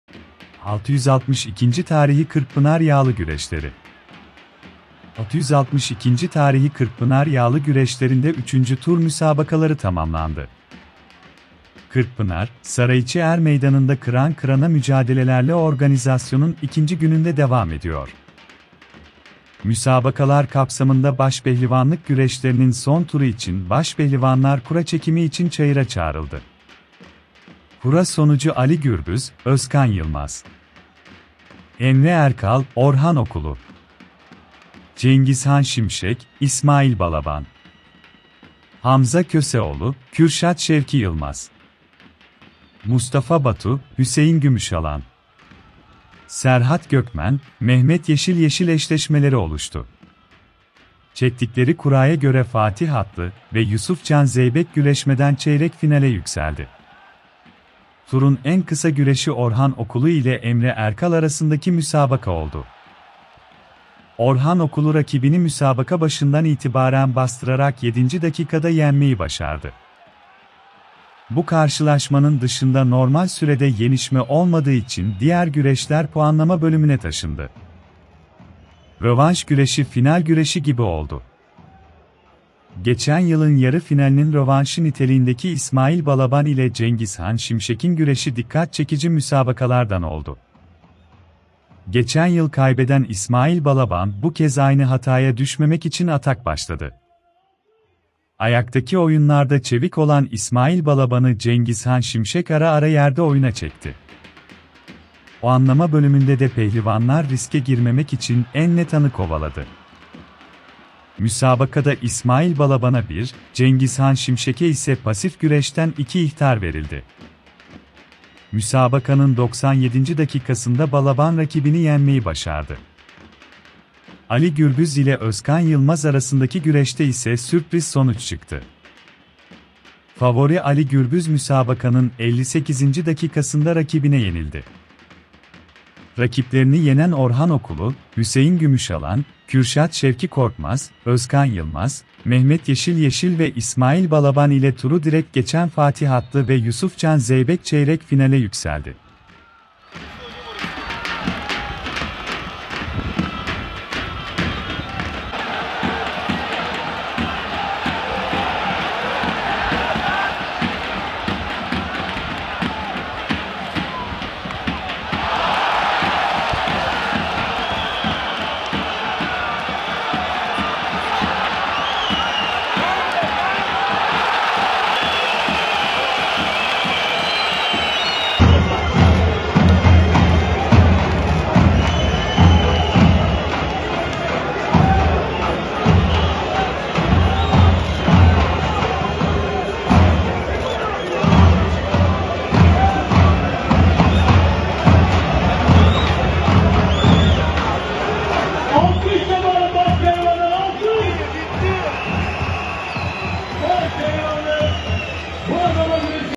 662. Tarihi Kırkpınar Yağlı Güreşleri
662. Tarihi Kırkpınar Yağlı Güreşleri
662. Tarihi Kırkpınar Yağlı Güreşleri'nde 3. tur müsabakaları tamamlandı.
Kırkpınar, Sarayiçi Er Meydanı'nda kıran kırana mücadelelerle organizasyonun ikinci gününde devam ediyor.
Müsabakalar kapsamında başpehlivanlık güreşlerinin son turu için başpehlivanlar kura çekimi için çayıra çağrıldı.
Kura sonucu Ali Gürbüz,Özkan Yılmaz,
 Emre Erkal,Orhan Okulu,
 Cengizhan Şimşek,İsmail Balaban,
Hamza Köseoğlu,Kürşat Şevki Yılmaz,
 Mustafa Batu,Hüseyin Gümüşalan,
Serhat Gökmen,Mehmet Yeşil Yeşil eşleşmeleri oluştu.
Çektikleri kuraya göre Fatih Atlı ve Yusuf Can Zeybek güreşmeden çeyrek finale yükseldi.
Turun en kısa güreşi Orhan Okulu ile Emre Erkal arasındaki müsabaka oldu. Orhan Okulu rakibini müsabaka başından itibaren bastırarak 7. dakikada yenmeyi başardı. Bu karşılaşmanın dışında normal sürede yenişme olmadığı için diğer güreşler puanlama bölümüne taşındı.

- Rövanş güreşi final güreşi gibi oldu
Geçen yılın yarı finalinin rövanşı niteliğindeki İsmail Balaban ile Cengizhan Şimşek'in güreşi dikkat çekici müsabakalardan oldu. Geçen yıl kaybeden İsmail Balaban bu kez aynı hataya düşmemek için atak başladı. Ayaktaki oyunlarda çevik olan İsmail Balaban'ı Cengizhan Şimşek ara ara yerde oyuna çekti. Puanlama bölümünde de pehlivanlar riske girmemek için en net anı kovaladı. Müsabakada İsmail Balaban'a bir, Cengizhan Şimşek'e ise pasif güreşten iki ihtar verildi. Müsabakanın 97. dakikasında Balaban rakibini yenmeyi başardı.
Ali Gürbüz ile Özkan Yılmaz arasındaki güreşte ise sürpriz sonuç çıktı. Favori Ali Gürbüz müsabakanın 58. dakikasında rakibine yenildi.
Rakiplerini yenen Orhan Okulu, Hüseyin Gümüşalan, Kürşat Şevki Korkmaz, Özkan Yılmaz, Mehmet Yeşil Yeşil ve İsmail Balaban ile turu direkt geçen Fatih Atlı ve Yusuf Can Zeybek çeyrek finale yükseldi.